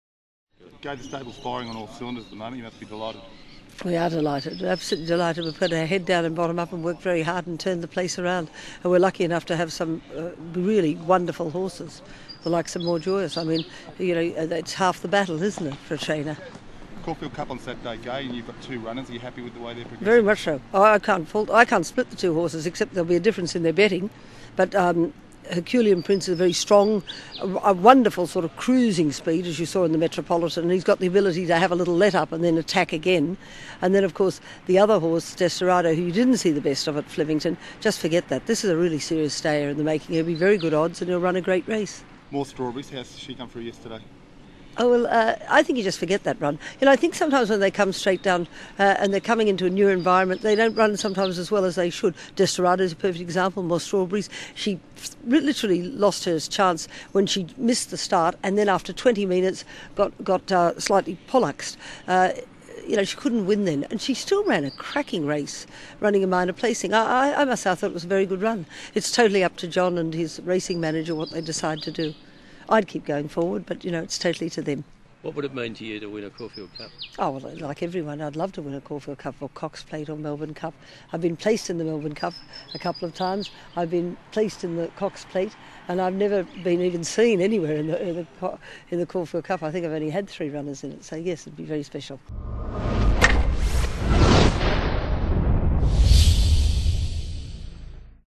Caulfield Cup thoughts from Gai Waterhouse
The "First Lady" of thoroughbred racing gives her thoughts on her stable stars and Cup and cox Plate chances